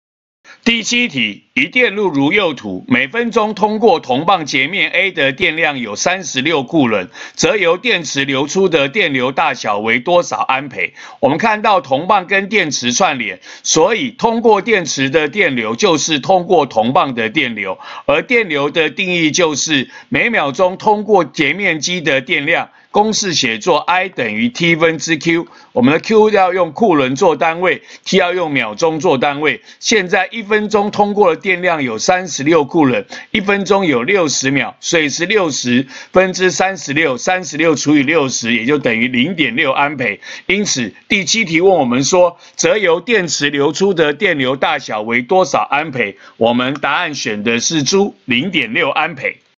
106學年上9年級理化三段第07題
給時間、電量問電流。